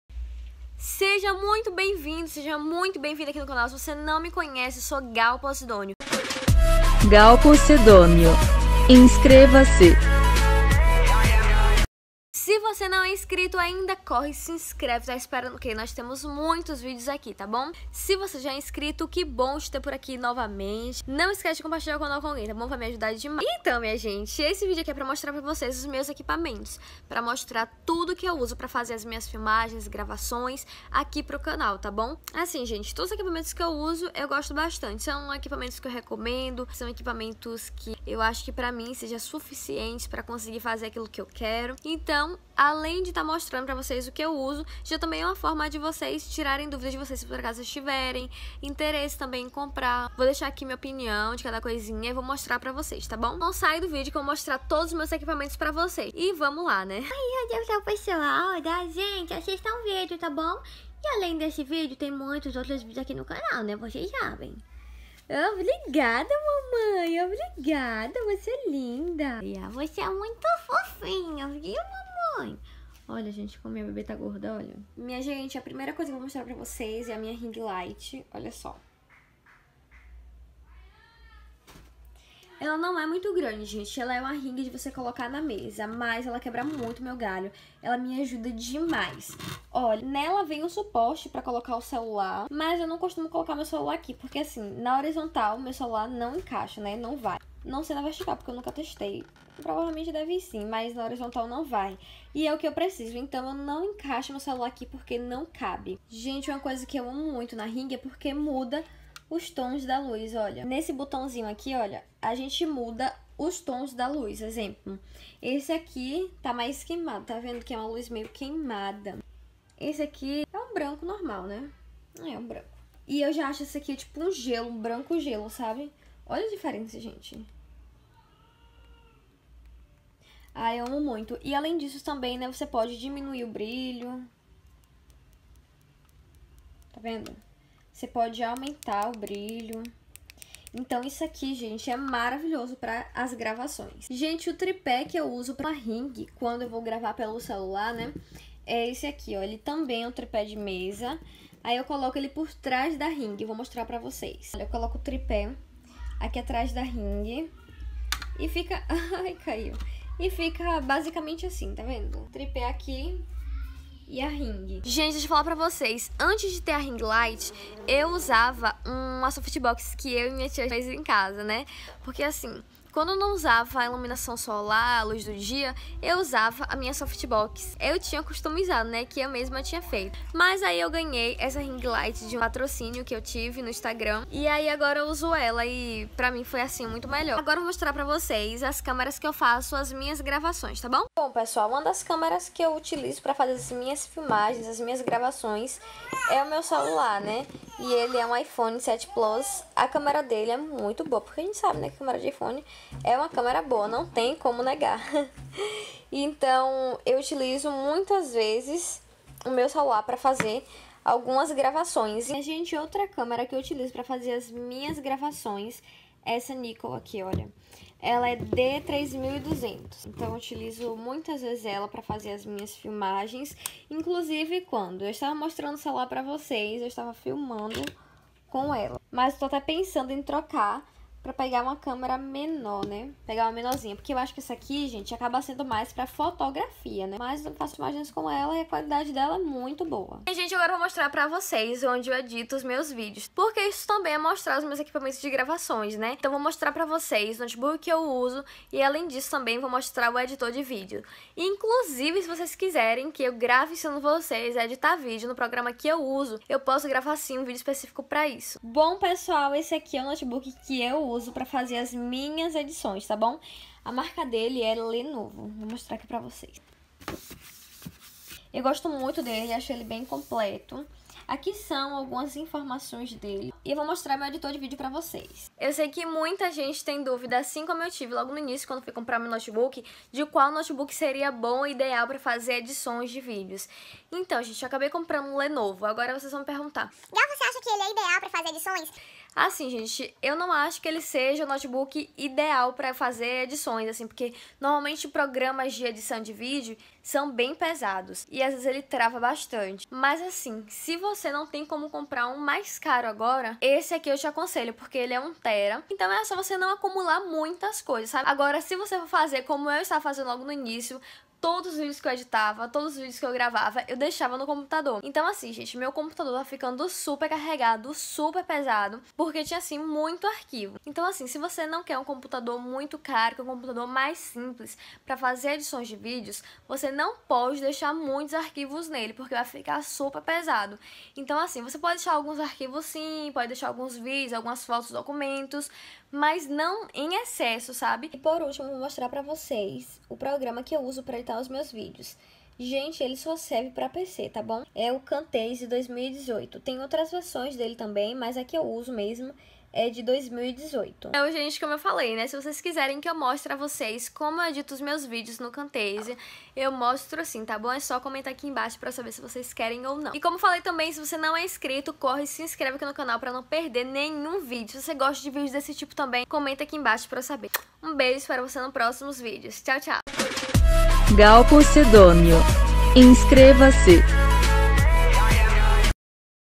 Como GRAVO meus vídeos!!! EQUIPAMENTOS, PROGRAMAS e DICAS!!! | Gal Possidônio
Vamos falar sobre equipamentos essenciais e dicas importantes e diretas pra quem está quer gravar vídeos!